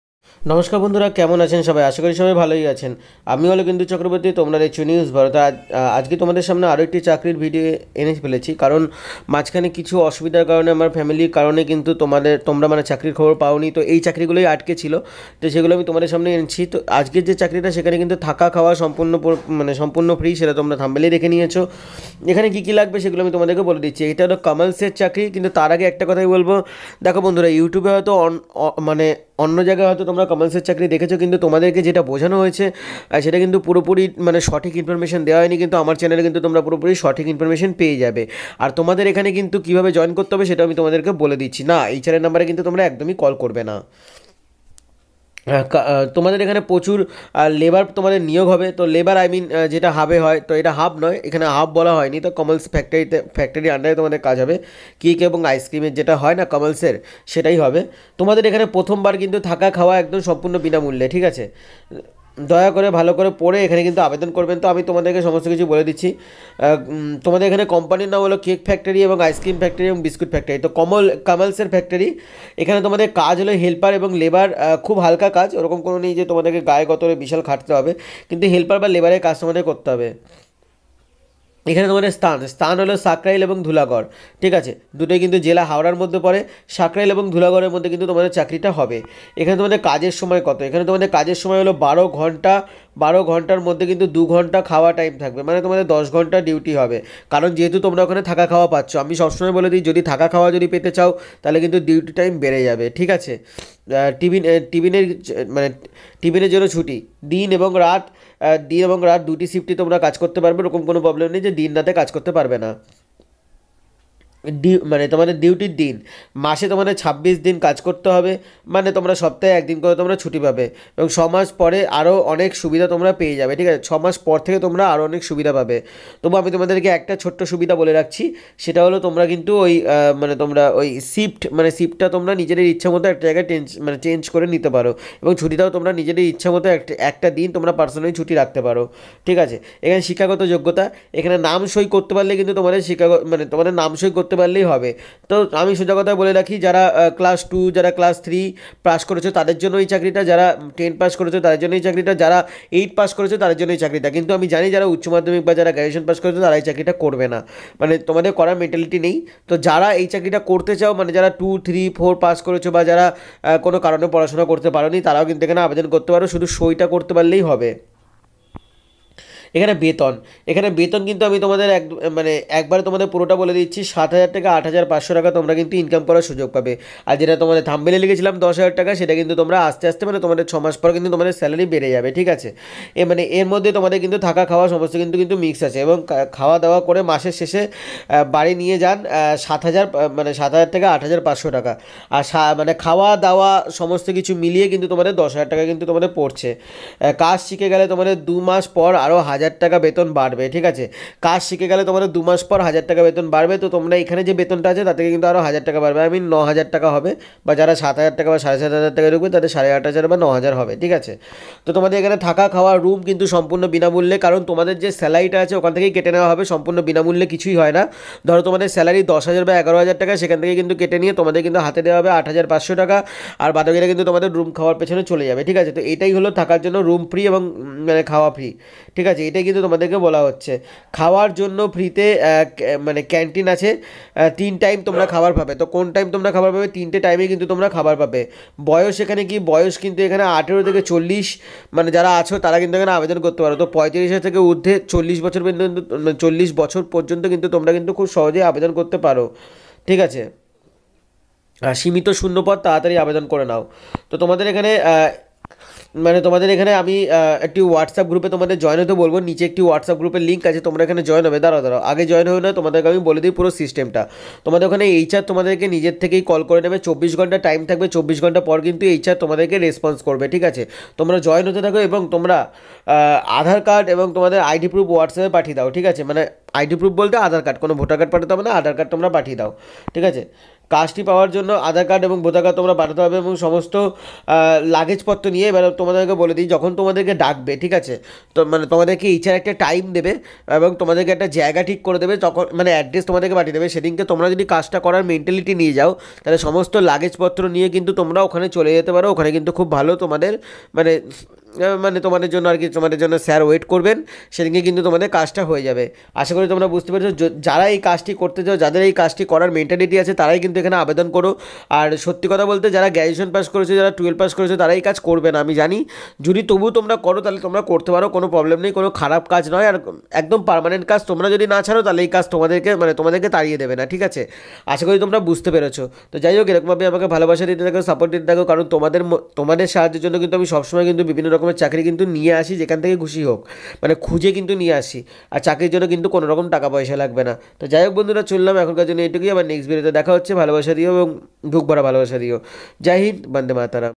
Kamal's Company Job 2024 || Private Job in Kolkata || Jobs for freshers || Kolkata job vacancy 2024
তাড়াতাড়ি জয়েন করে নিন চাকরিটি করার জন্য

ডাইরেক্ট জয়নিং  করিয়ে দেওয়া হবে
*হেল্পলাইন

*🌟 🌟 🔊🔊প্রচুর লেবার/   শ্রমিক  নামী দামী কোম্পানীতে🌟🌟*

*এই প্রথমবার থাকা খাওয়া ফ্র সাথে ভালো বেতন*

*👍দয়া করে ভালো করে দেখে আবেদন করুন।*

*💥কোম্পানির নাম ==} কেক ফ্যাক্টরি ও আইসক্রীম ফ্যাক্ট ও বিস্কুট

*💥 কাজ ==} হেল্পার, লেবার খুব হালকা কাজ*
যারা একটু খাটতে পারে তাদের বেতন আরো বেশি।

*💥 স্থান ==} ধুলাগড়,  ও  সাঁকরাইল*(জেলা হাওড়া)

💥 কাজের সময় ==} ১২ ঘন্টা এরমধ্যে 2 ঘন্টা খাওয়া  ও টিফিন এর ছুটি ( দিন এবং রাত দুটি শিফট এ কাজ করতে হবে )

💥 ডিউটি দিন ==} মাসে 26 দিন ডিউটি( No work no pay) 6মাস পরে আরও অনেক সুযোগ-সুবিধা পাবেন।

💥 শিক্ষাগত যোগ্যতা ==} নাম সই করতে পারলেই হবে।

*💥 বেতন ==} ☀️হেল্পার এবং লেবার :- 240 to 265 টাকা/প্রতিদিন*
থেকে ও খাওয়া-দাওয়া করে মাসের শেষে বাড়ি নিয়ে যান
Rs 7000 to 8500 টাকা ইনকাম করুন। এই সুযোগ  প্রথমবার।

(কাজ শিখে গেলে বেতন বাড়বে 2 মাস পরে 1000 টাকা বেতন বাড়বে।)

*💥 থাকার জন্য রুম ফ্রী  টাকা পয়সা লাগবে না।*

*😊খাওয়ার জন্য ফ্রিতে ক্যান্টিন আছে দিনে 3 টাইম খাওয়া একদম ফ্রি*🙏

💥 বয়স ==} ১৮ থেকে 40 বছর
*💥 সীমিত শূন্যপদ  কোম্পানি তে তাই তাড়াতাড়ি করুন। আগে এলে আগে সুযোগ পাবেন 🏃‍♀️🏃‍♀️🏃‍♀️*

*যোগাযোগ 📞
আমাদের এই নাম্বারে আপনার Aadhaar card আইডি প্রুফ হোয়াটসঅ্যাপে পাঠিয়ে নিচে লিখে দেবেন  আইসক্রিম ফ্যাক্টরি এবং কল করুন।

(কাজটি পাওয়ার জন্য আপনাকে আপনার আধার কার্ড ও ভোটার কার্ড  ও দুই কপি ছবি নিয়ে আসতে হবে।)

*🤝 সমস্ত লাগেজ পত্র নিয়ে আসতে হবে ডাইরেক্ট জয়েন করানো হবে কোন রকম ইন্টারভিউ হবে না।*
*সমস্ত লাগেজ পত্র নিয়ে একবারে আসতে হবে ডাইরেক্ট জয়েন করানো হবে।*

 *(সাঁকরাইল ইন্ডাস্ট্রিয়াল পার্ক)
 (জেলা হাওড়া)*
*প্রত্যেককে /সাঁকরাইলের ইন্ডাস্ট্রিয়াল পার্ক* *বাসস্টপে নেমে ফোন করে নেবেন। ধুলাগর টোল প্লাজার কাছে।*

👉 *আপনার ভালো লাগবে এই বিজ্ঞাপনটির সকলের কাছে শেয়ার করতে পারেন যাঁদের কাজের দরকার আছে এবং কাজ করার মানসিকতা আছে।*

*কাজের জন্য কোনরকম চার্জেস লাগেনা।*ও কোন রকম টাকা পয়সা দেবেন না।*

Genuine manpower and Consaltent also welcome

News Bharat team ♥️♥️